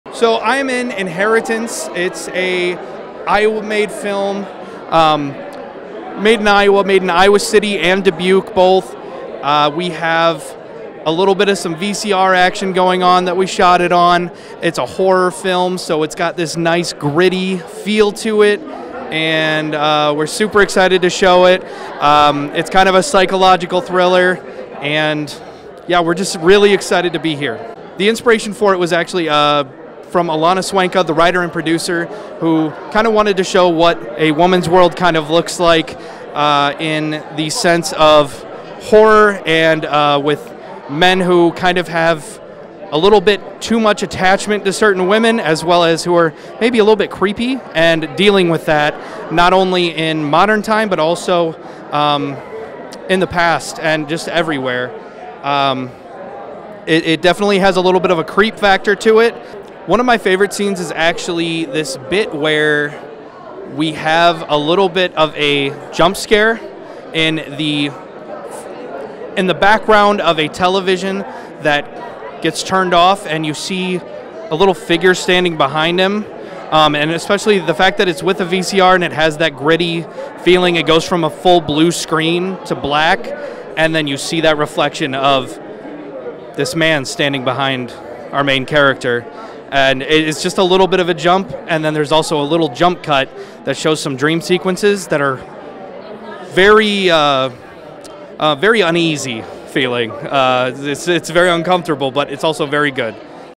Matty Weiland discusses "Inheritance"
Matty Weiland speaks about his film "Inheritance" at Julien Dubuque International Film Festival on Saturday, April 28, 2024.